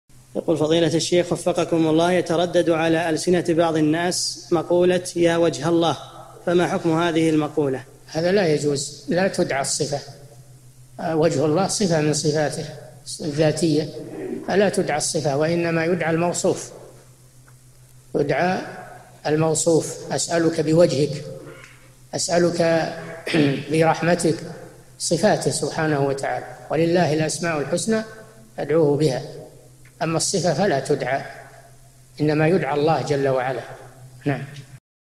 [472 /1350] يتردد على ألسنة بعض الناس مقولة:" يا وجه الله" فما حكم هذه المقولة؟ الشيخ صالح الفوزان
-[472 /1350] السؤال: يتردد على ألسنة بعض الناس مقولة:" يا وجه الله" فما حكم هذه المقولة؟
 الجواب: هذا لا يجوز، لا تُدعى الصفة، وجه الله صفة من صفاته الذاتية، فلا تُدعى الصفة، وإنما يُدعى الموصوف، أسألك بوجهك، أسألك برحمتك، صفاته سبحانه وتعالى ، ﴿وَلِلَّهِ الْأَسمَاءُ الْحُسنَى فَادْعُوهُ بِهَا﴾ [الأعراف: 180]، أما الصفة فلا تُدعى، إنما يُدعى الله عز وجل.
للاشتراك في قناة الشيخ صالح الفوزان
أسئلة وأجوبة من شرح قاعدة جليلة ( 1350 ) مقطع

للاستفسار والإقتراحات والدعم
رئيس الفريق
لمشروع كبار العلماء